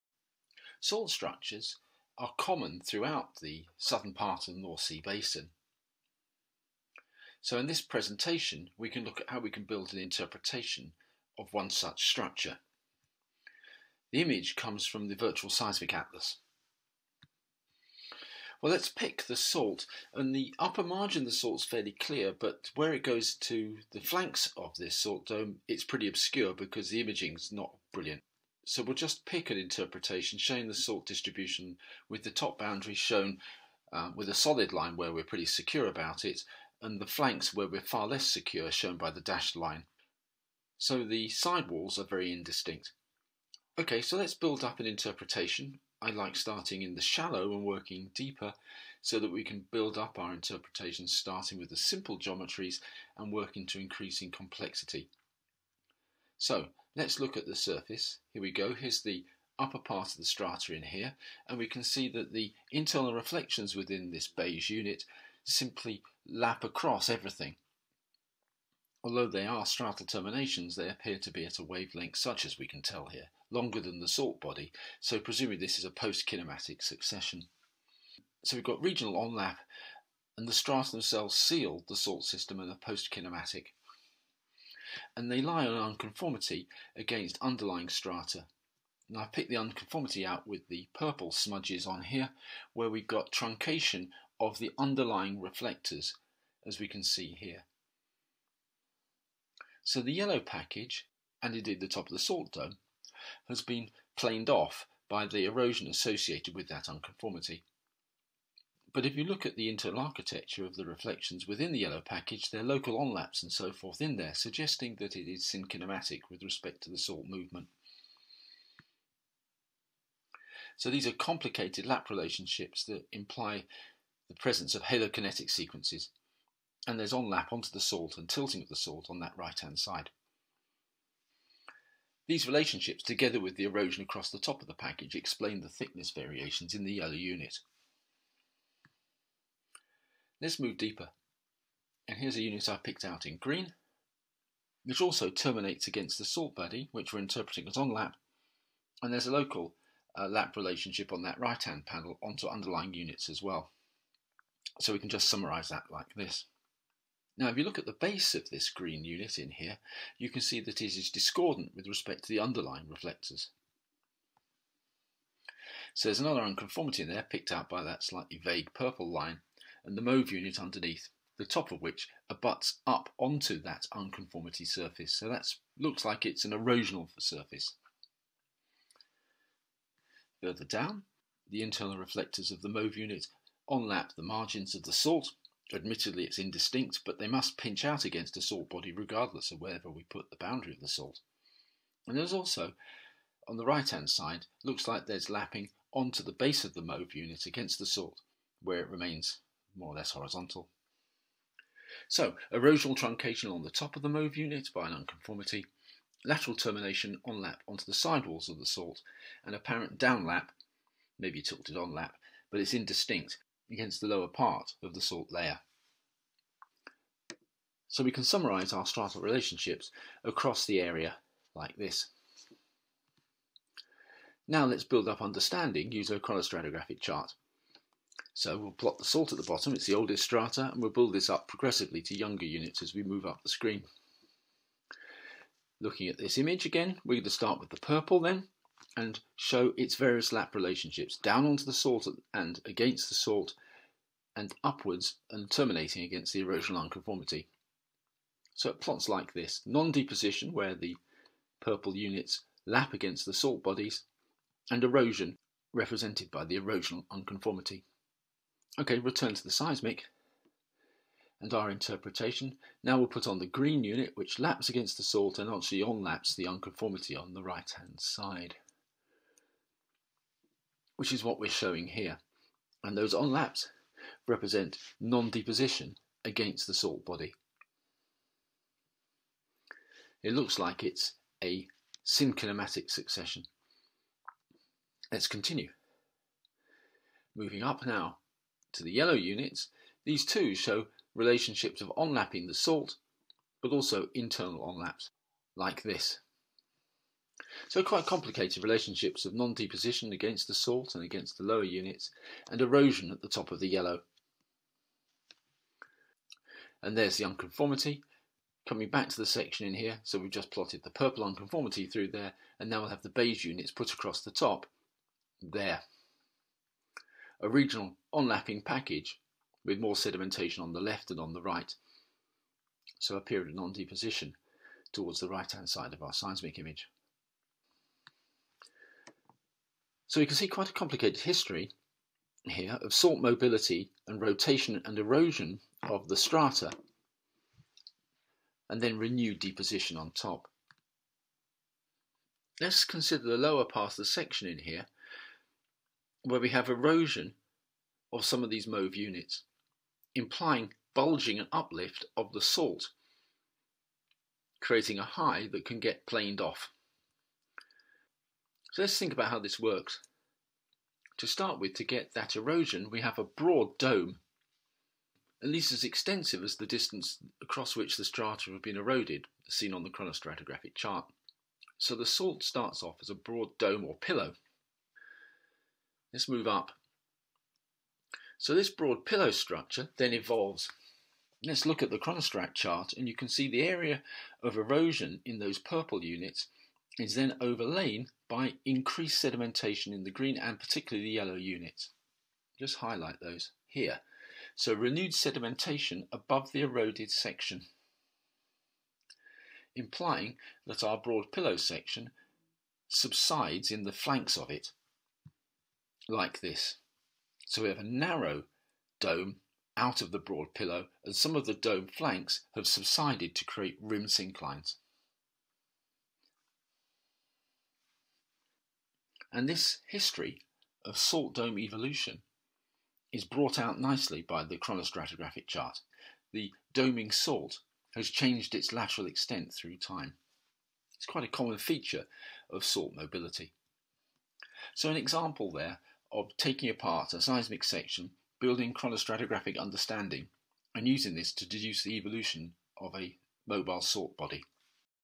Interpreting salt 2
Part of The shear Zone channel. This is a short demonstration of an interpretation of a seismic reflection profile across a salt structure from the North Sea.